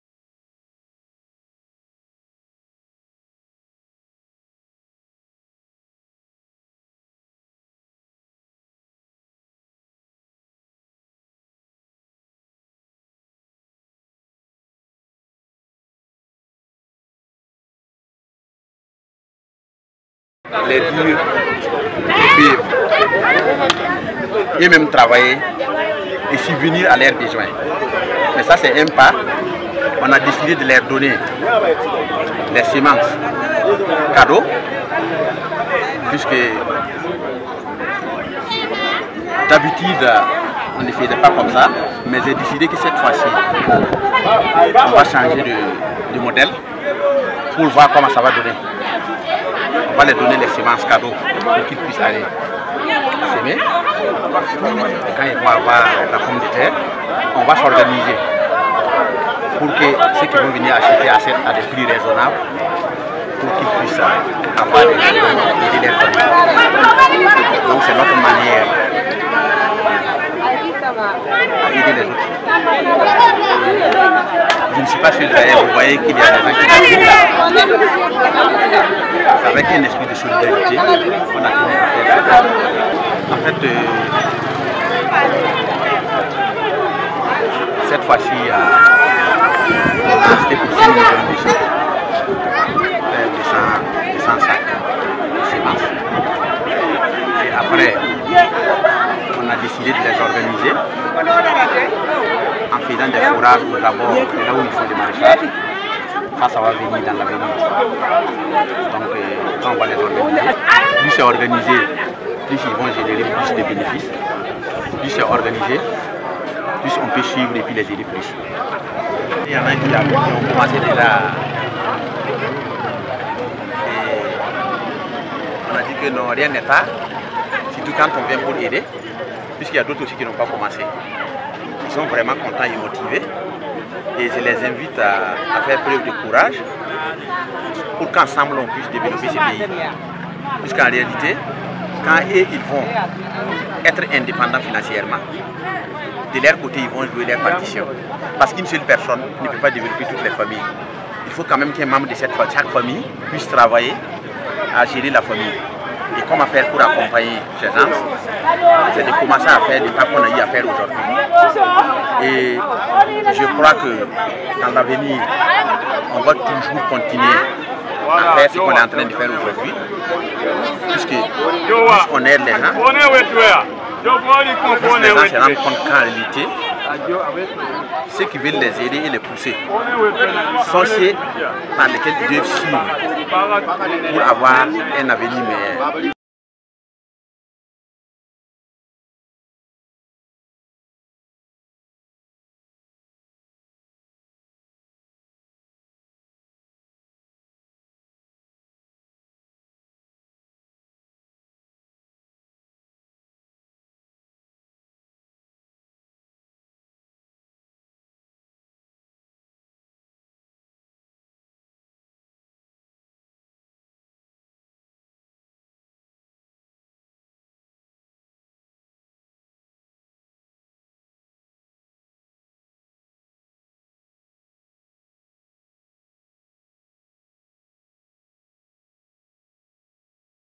Don de semences de pomme de terre à des producteurs du Yatenga
Au Yatenga , la culture maraichère est en vogue dans la saison sèche.ils sont nombreux les producteurs qui ont des difficultés pour se procurer les semences afin d'entamer leurs activités . L'opérateur économique Ousséni Nacanabo dit Yoro a volé au secours de ces derniers en leur dotant 100 sacs de semence de pomme de terre . La cérémonie de remise a eu lieu le samedi 29 décembre au siégè de l'association Burkina Vert